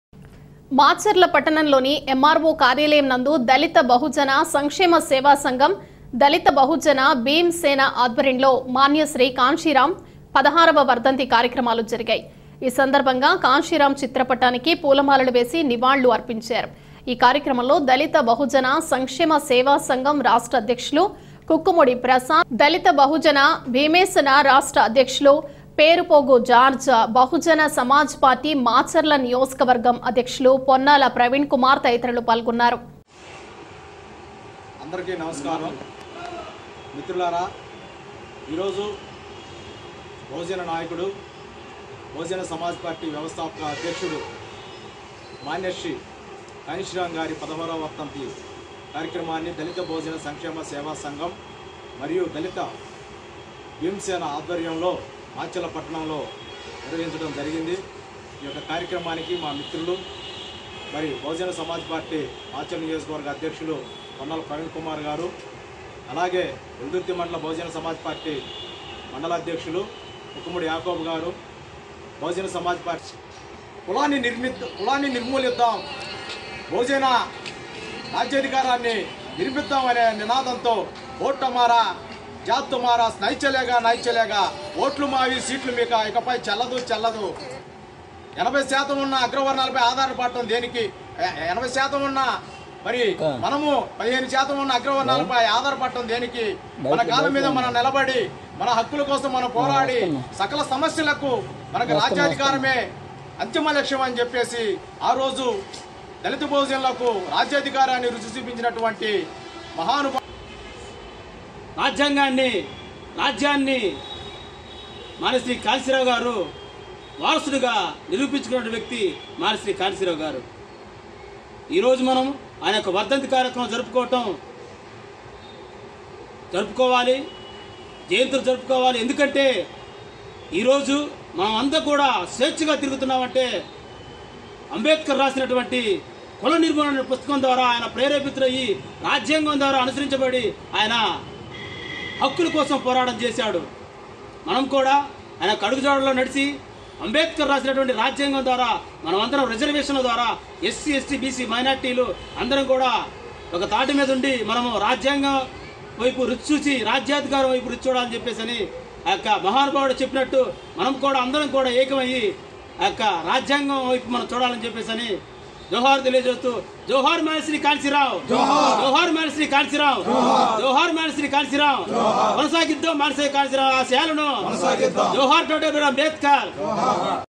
మాచర్ల పట్టణంలోని ఎమ్మార్వో కార్యాలయం నందు మాన్యశ్రీ కాన్సిరామ్ 16 వ వర్ధంతి కార్యక్రమాలు
మాచర్ల పట్టణంలోని ఎమ్మార్వో కార్యాలయం నందు దళిత బహుజన సంక్షేమ సేవా సంఘం,దళిత బహుజన భీమ్ సేన ఆధ్వర్యంలో మాన్యశ్రీ కాన్సిరామ్ 16 వ వర్ధంతి కార్యక్రమాలు జరిగాయి. ఈ సందర్భంగా కాన్షిరామ్ చిత్రపటానికి పూలమాలలు వేసి నివాళులు అర్పించారు. ఈ కార్యక్రమంలో దళిత బహుజన సంక్షేమ సేవా సంఘం రాష్ట్ర అధ్యక్షులు కుక్కమూడి ప్రసాద్, దళిత బహుజన భీమ్సేన రాష్ట్ర అధ్యక్షులు పేరు పోగు జార్జి, బహుజన సమాజ్ పార్టీ మాచర్ల నియోజకవర్గం అధ్యక్షులు పొన్నాల ప్రవీణ్ కుమార్ తదితరులు పాల్గొన్నారు.